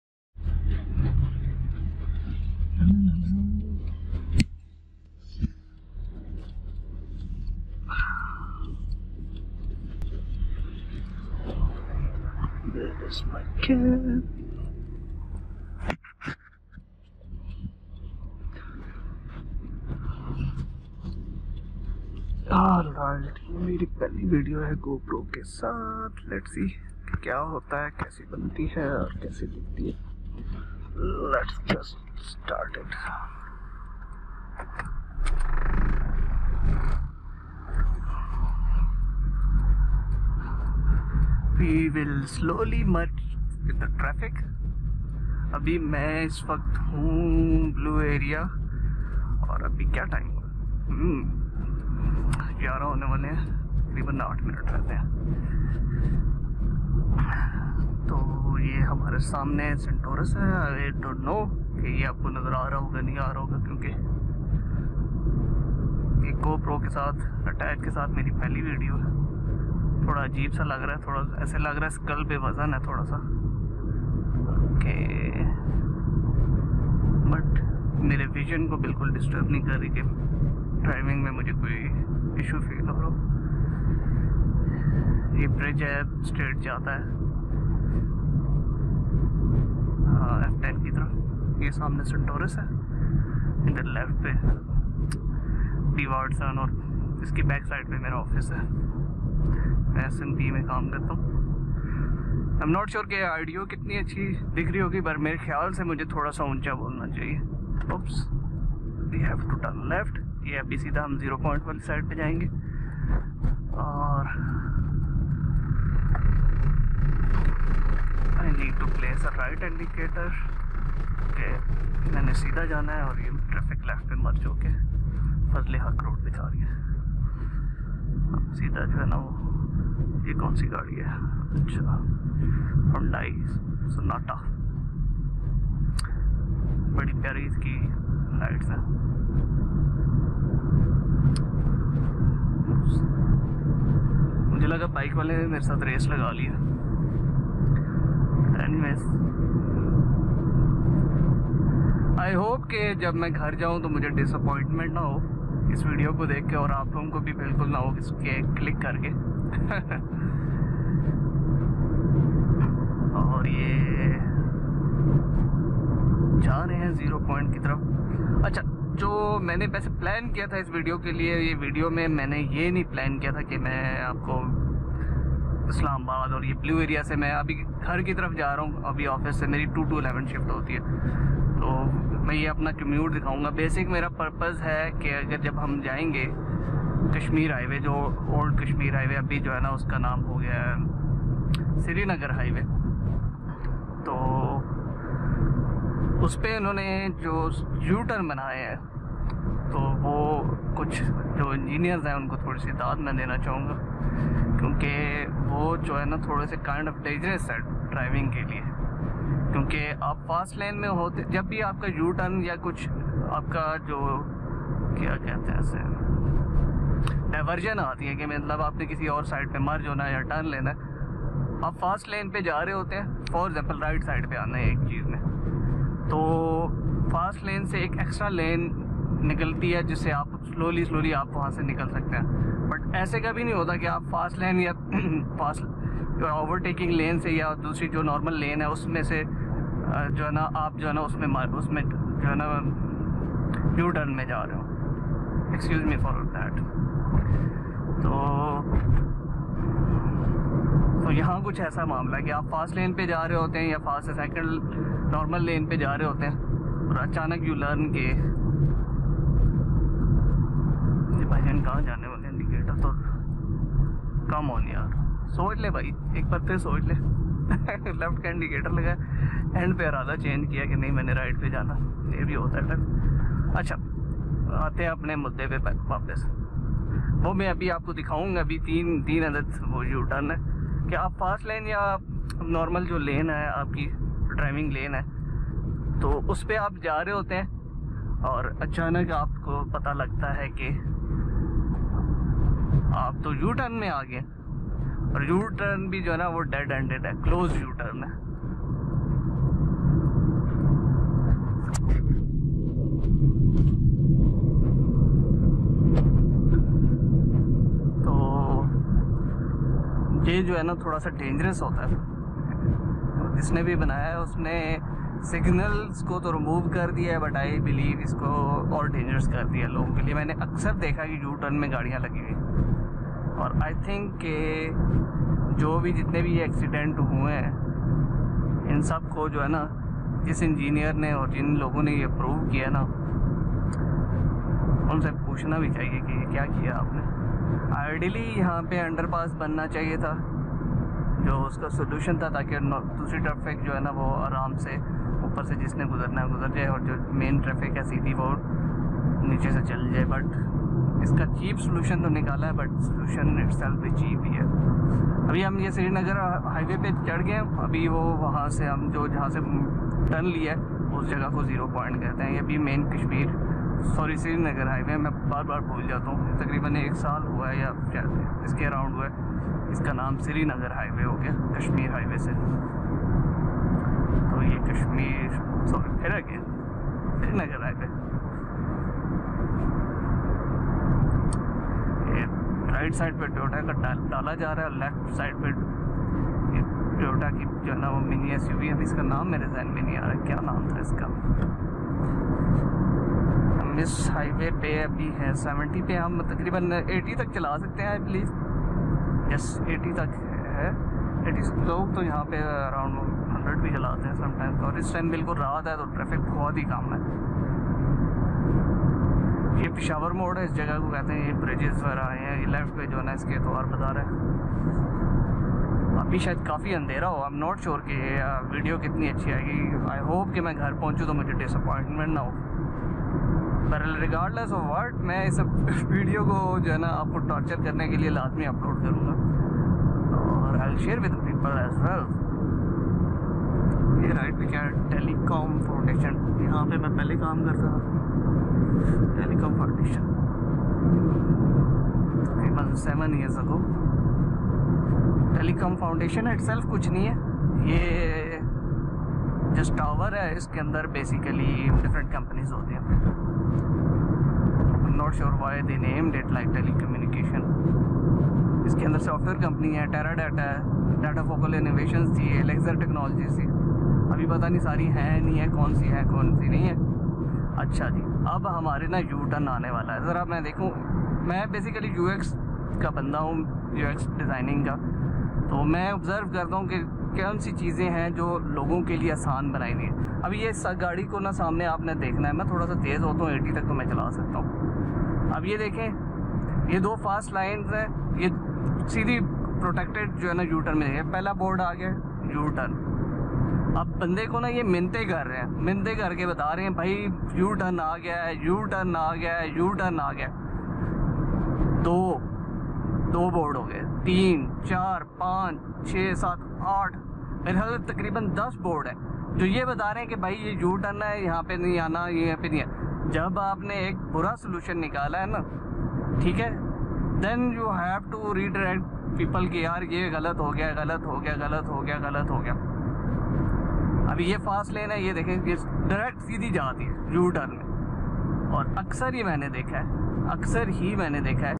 POV Driving in Islamabad Part 01
In this video clip one I have travelled in the Islamabad and shared some opinions about current affairs and road situation.